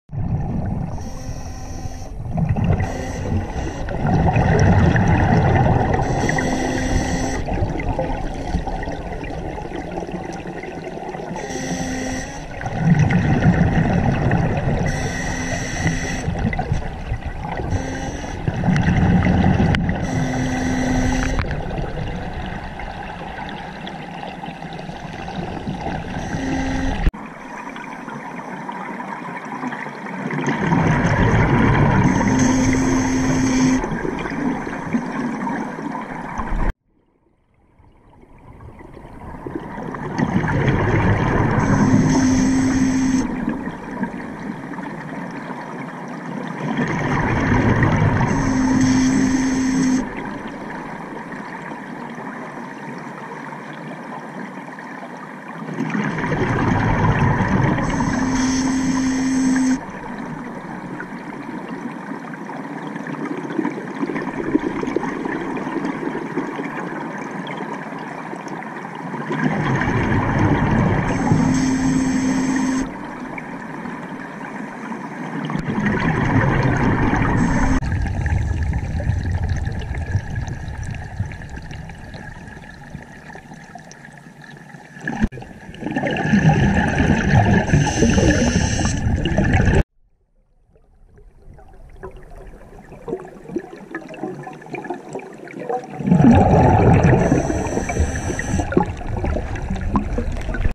Water Operations Unit - Allendale
GoPro footage from the Allendale Sinkhole during Water Operations Unit training in the south-east of South Australia.